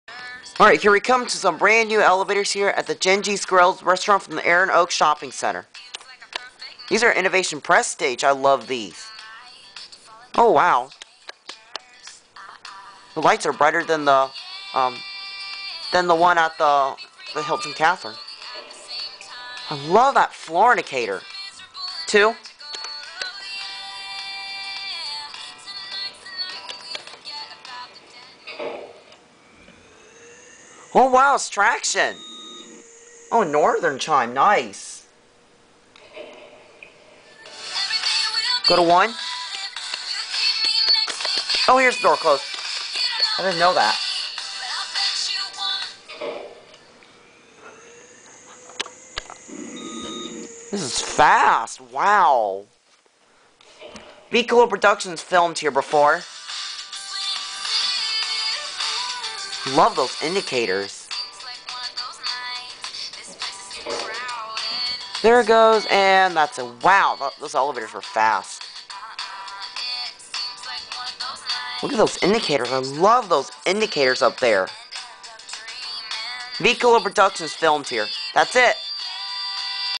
Animated: New Kone MRL Traction Elevators at Gengis Grill, Erin Oaks Shopping Center
Elevationville. AWESOME ELEVATORS! Vkolaproductions has filmed here and special thanks to him for guiding me here!